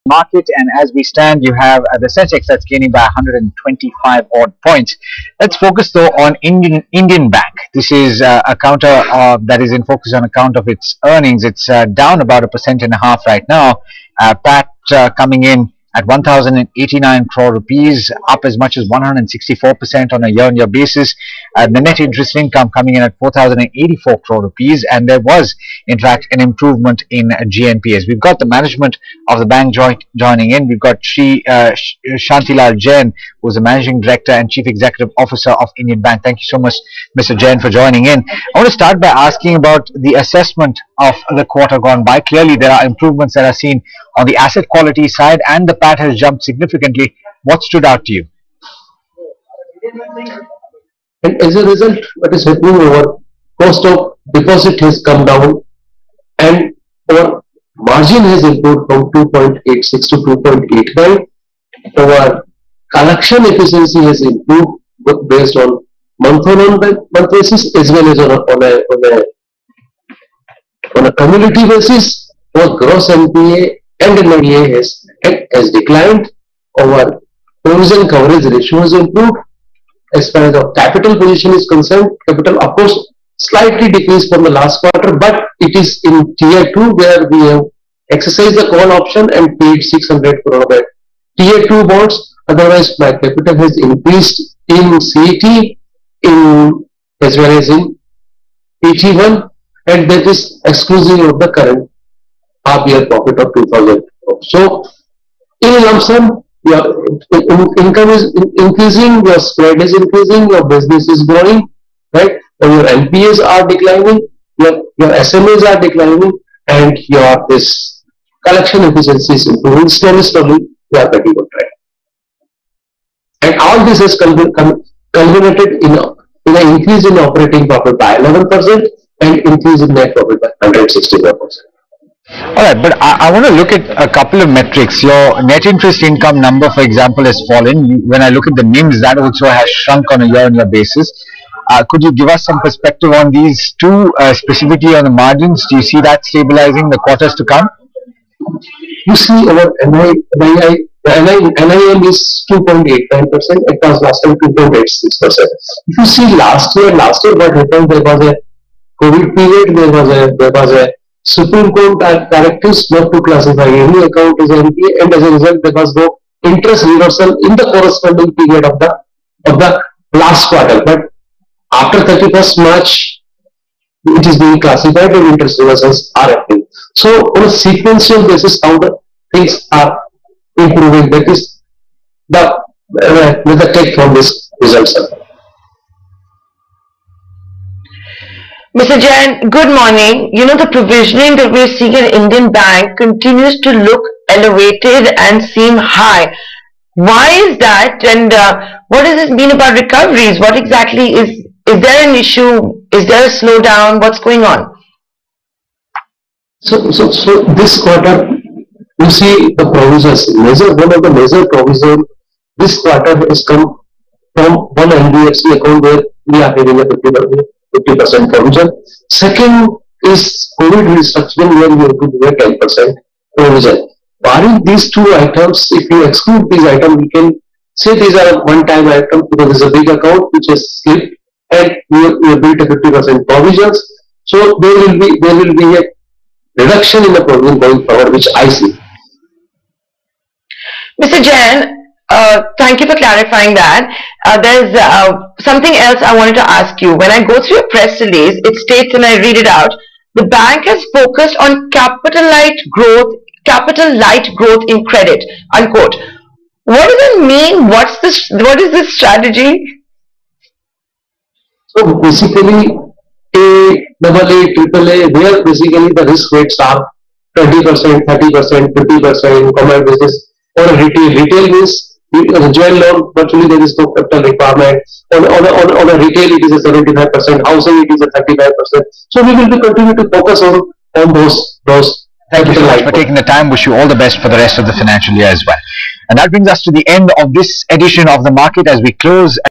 Indian Bank, MD&CEO talks on Q2, FY 22 results.
Our MD & CEO, Shri. Shanti Lal Jain, speaks to ET Now (Earnings) about Indian Bank’s performance in Q2, FY 2021-22. Watch the video to know more.